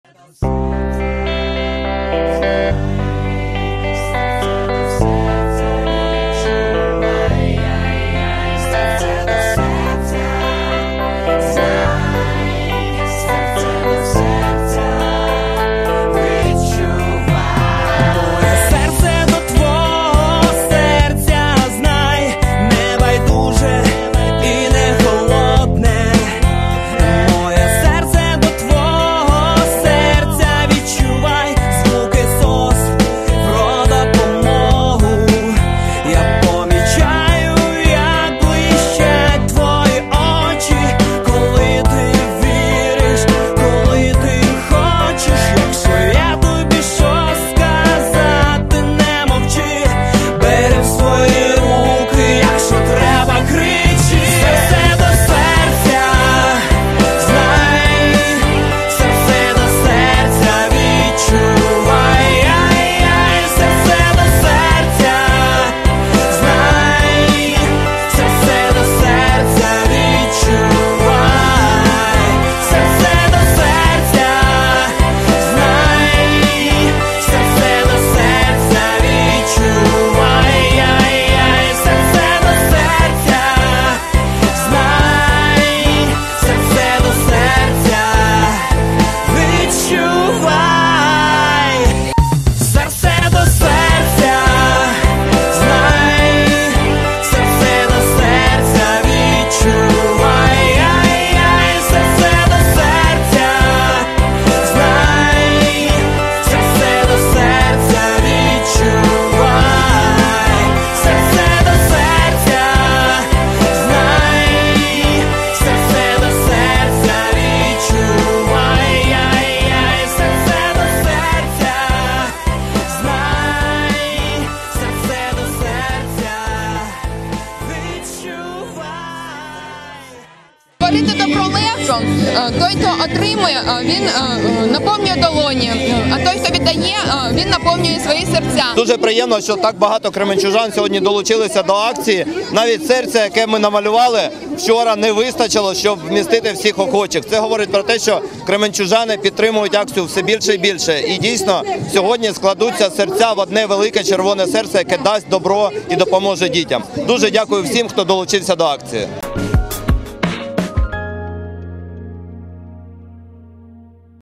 ФІНАЛ АКЦІЇ “СЕРЦЕ ДО СЕРЦЯ 2018”. ФЛЕШМОБ
НА ПЛОЩІ ПЕРЕМОГИ КРЕМЕНЧУЖАНИ ПРОВЕЛИ ВРАЖАЮЧИЙ ТА ЯСКРАВИЙ ФЛЕШ-МОБ «ЖИВЕ СЕРЦЕ»